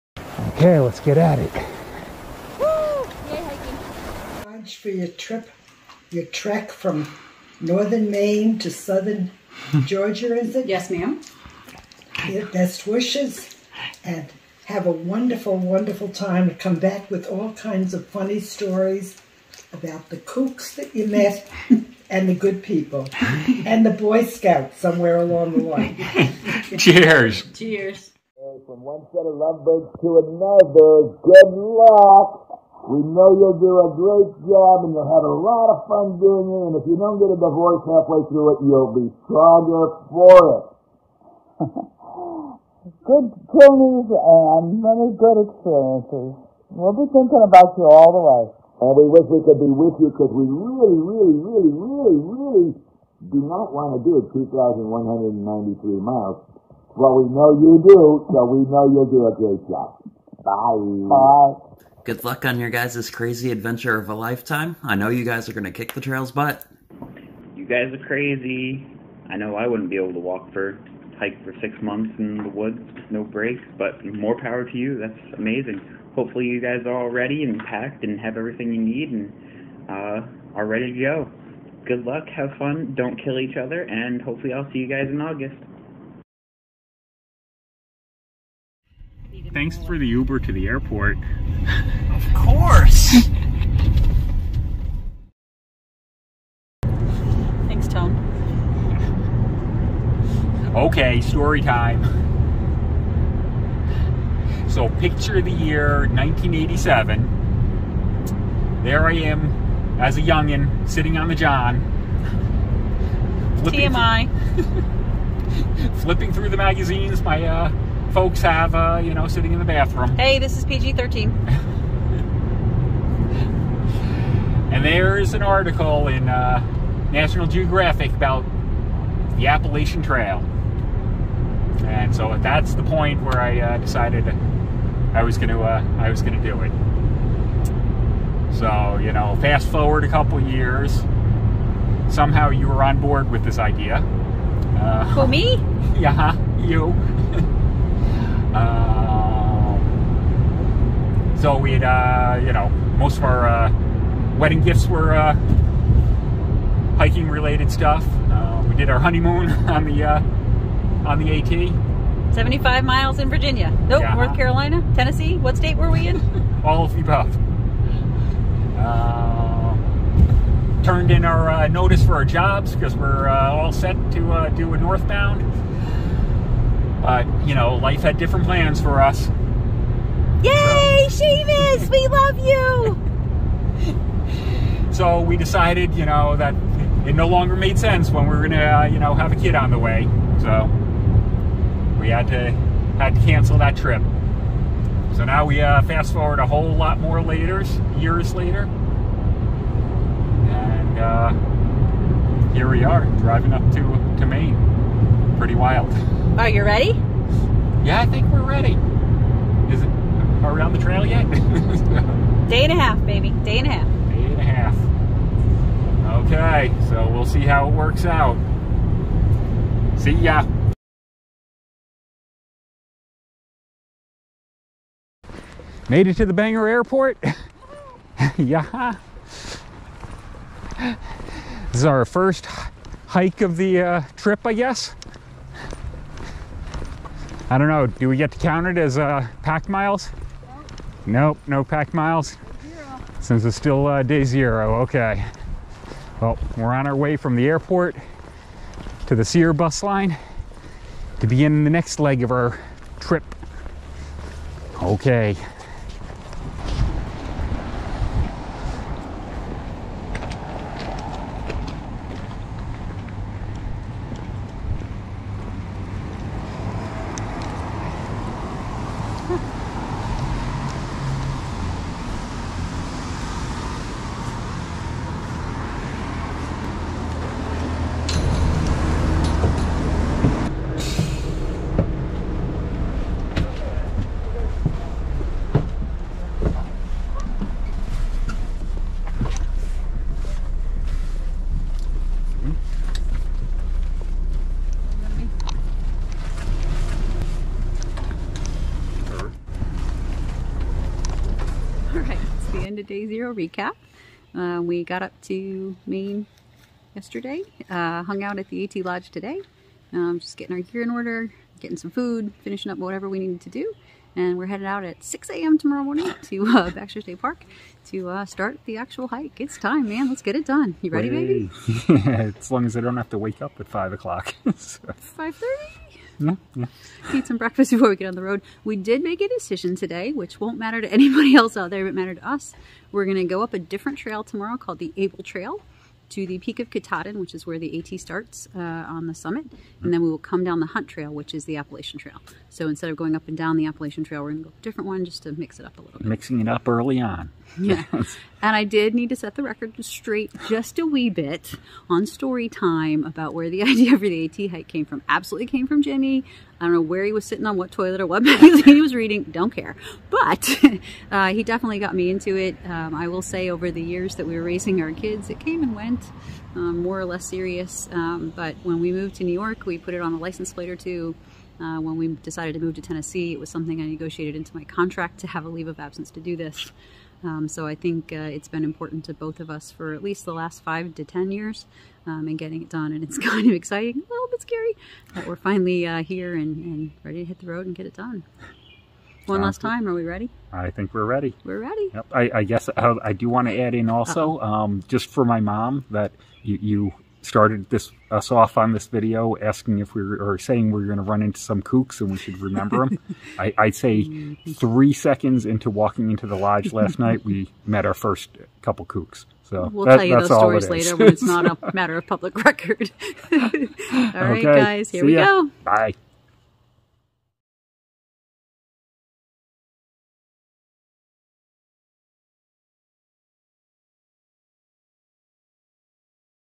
SOBO'21 - Day 0: Millinocket, ME
Day 0 of our 2021 Southbound Thru-Hike!

After visiting family in Virginia and New York, we travelled by car+bus+shuttle to the Appalachian Trail Lodge in Millinocket, Maine.

No hiking in this video, just our final prep before heading into Baxter State Park and stepping off on June 2.

We are expecting radio silence for the next ~10 days as we make our way south through the 100 mile wilderness, so no new videos will post until we make it through that stretch. Wish us luck!!